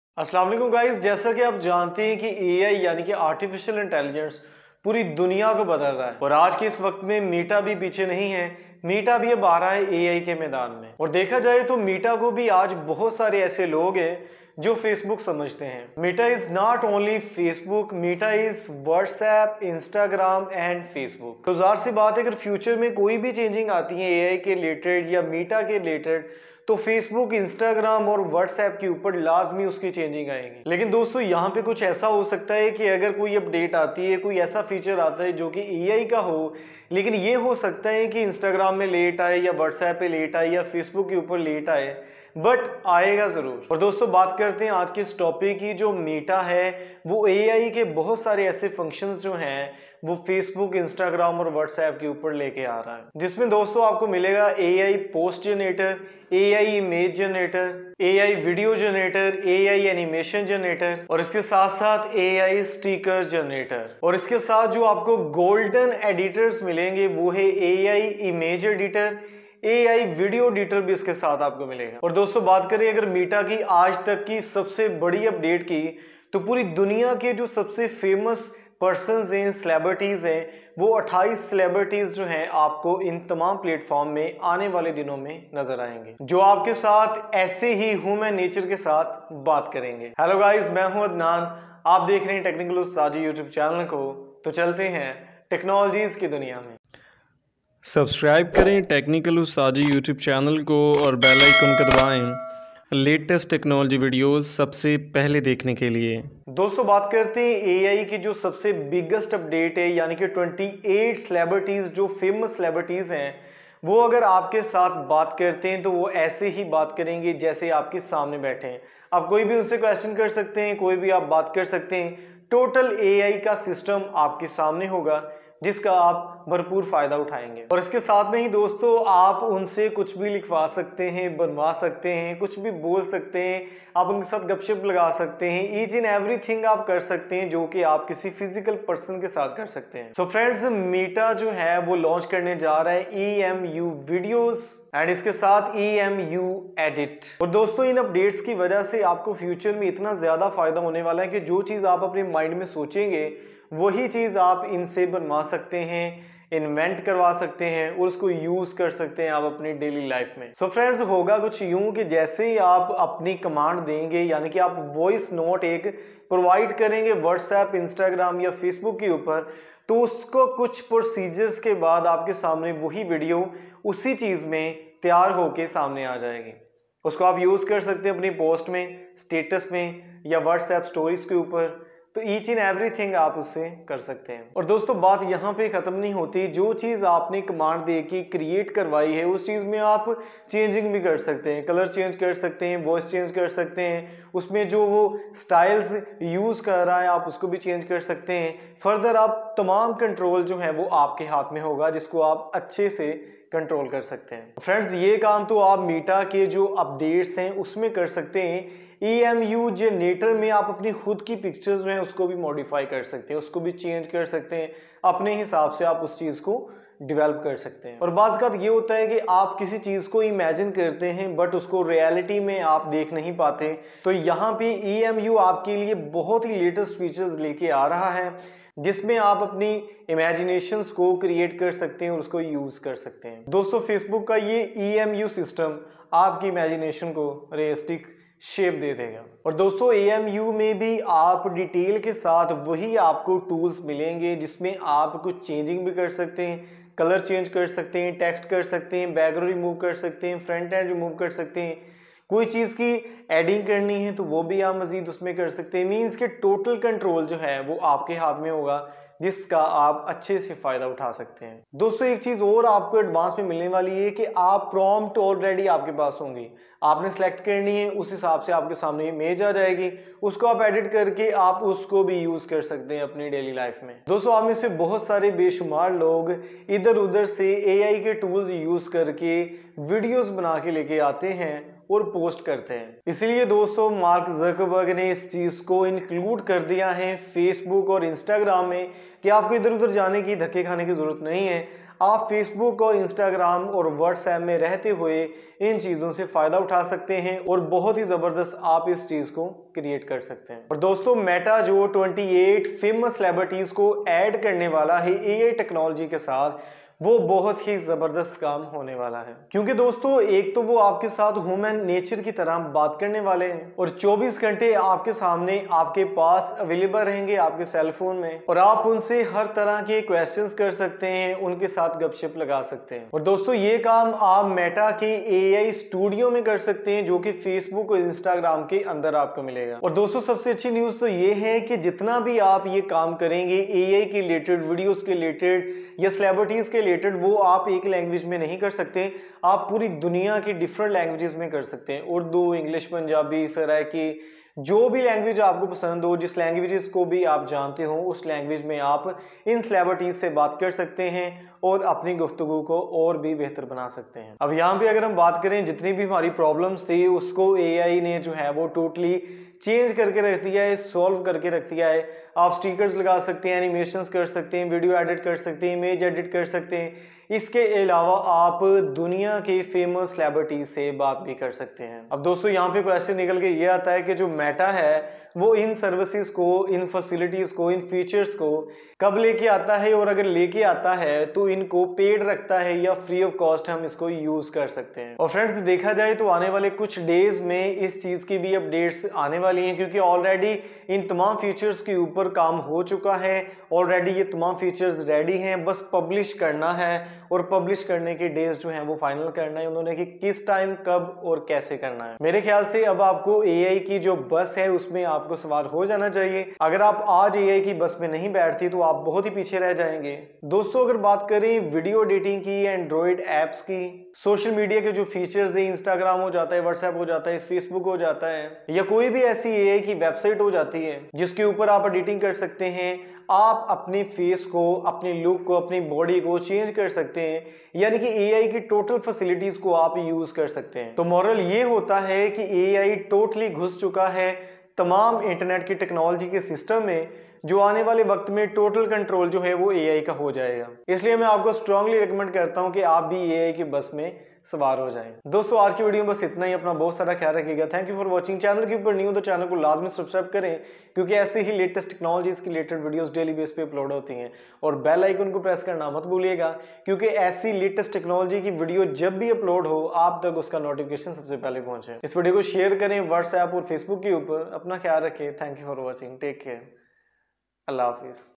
Meta Unveils Next-Gen Powerful AI Video and Image Editing Tools
Hey Guys! So Meta announced two new AI-powered creative tools that will soon be available on Facebook and Instagram and maybe soon for WhatsApp. Dubbed Emu Video and Emu Edit, the features leverage Meta’s Emu generative AI model to enable easy video and image editing by using text prompts.

About : Technical Ustadji is a YouTube Channel, where you will find technological videos in Urdu/Hindi, New Video is Posted After day :)